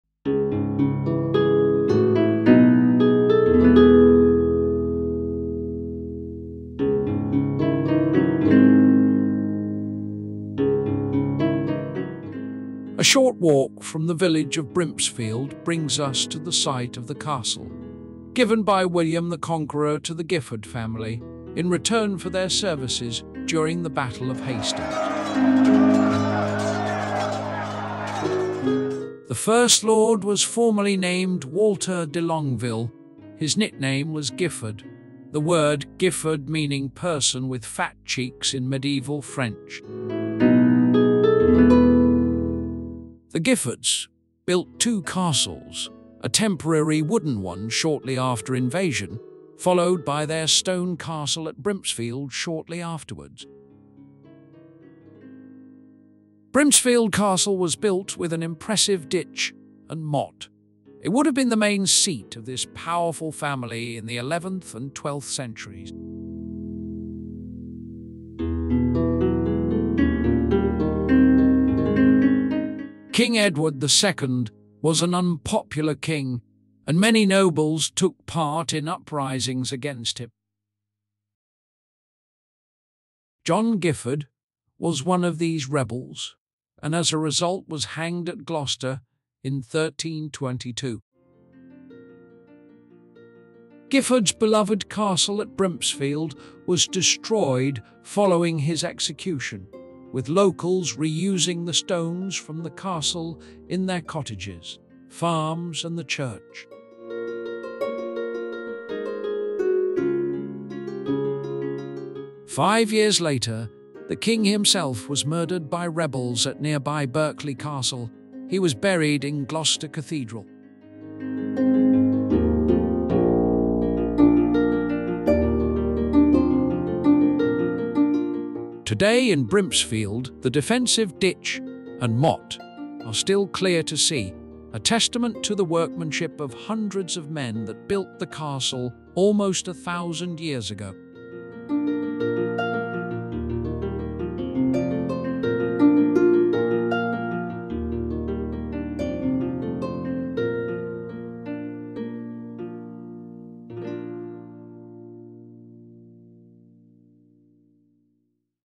Brimpsfield Castle
The lost castle at Brimpsfield, Gloucestershire, Cotswolds. Once the seat of the powerful Gifford family, now only the earthworks remain. This short film tells the story of the lost castle at Brimpsfield.